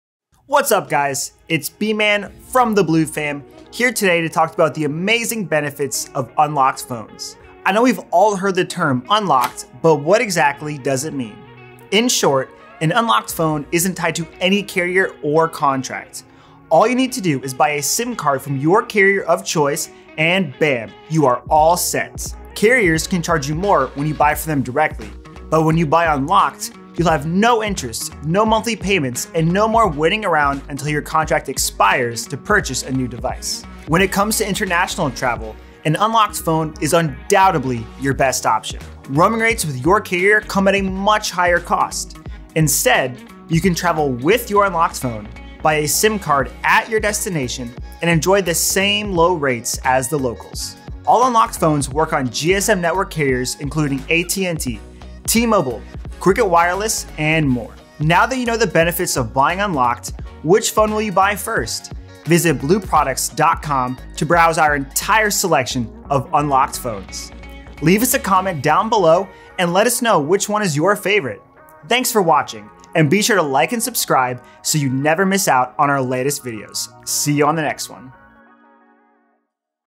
Why unlocked phones are a GREAT option
Unlocked phones are great options for those looking for more flexibility and control over their devices. Unlocked phones allow you to use any compatible SIM card from any carrier, so you can switch carriers or use a local SIM card when traveling abroad. They also give you the freedom to choose more affordable plans and services, as well as access to apps that may not be available on certain carriers. Additionally, unlocked phones are generally more affordable than locked models, and can be used on networks that are incompatible with locked phones.